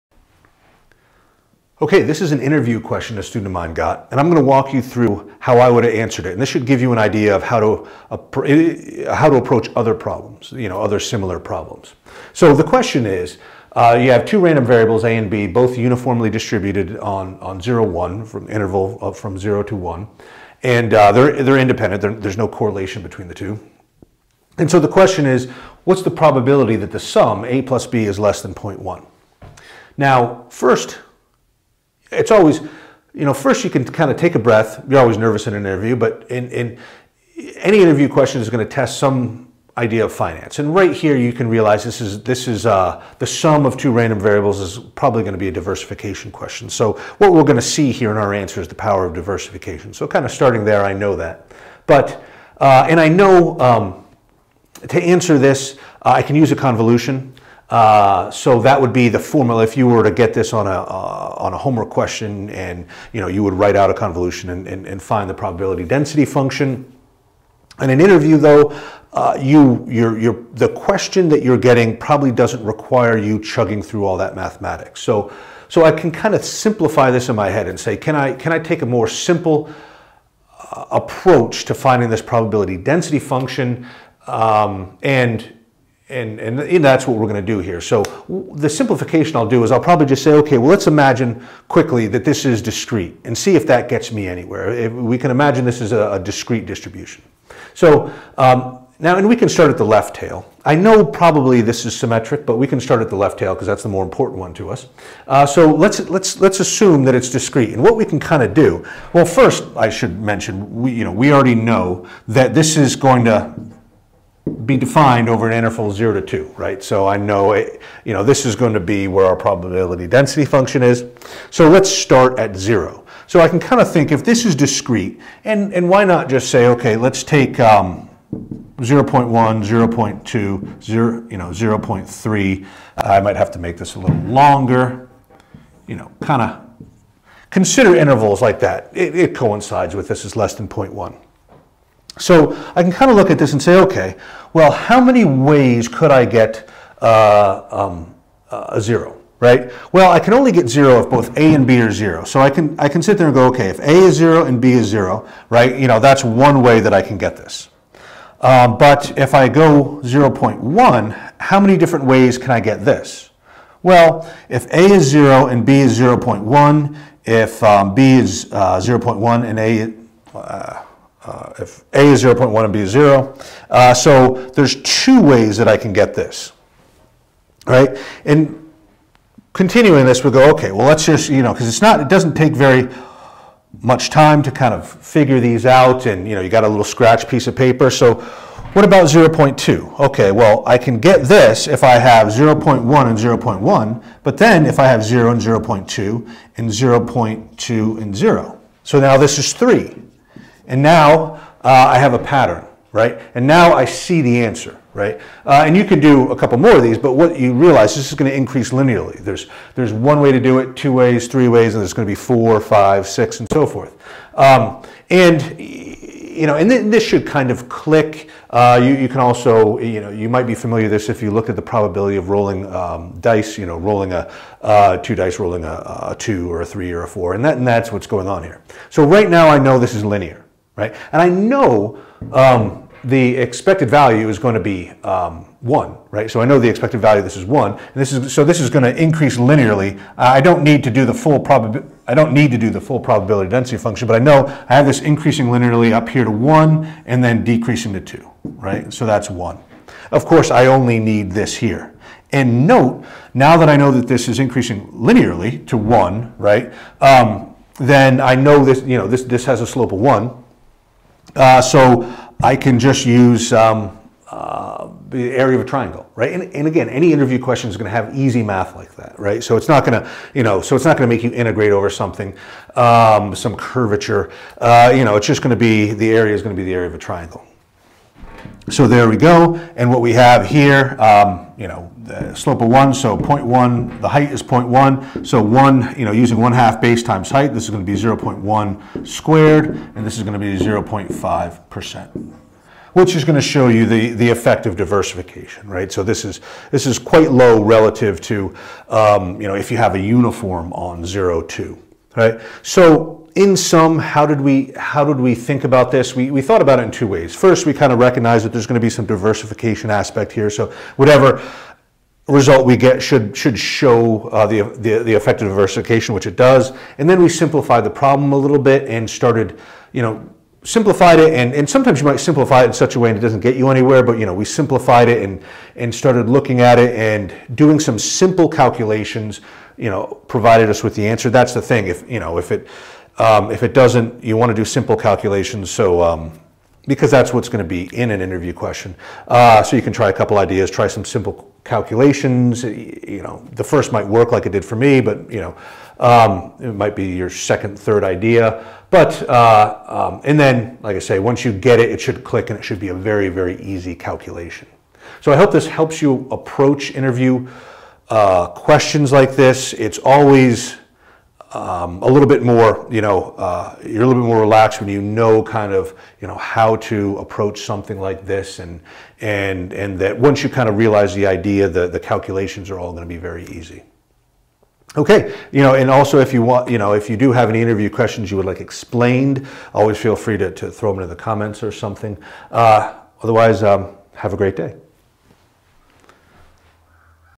Interview Question: Sum of Two Uniform Random Variables
The solution with advice on how to approach interview questions.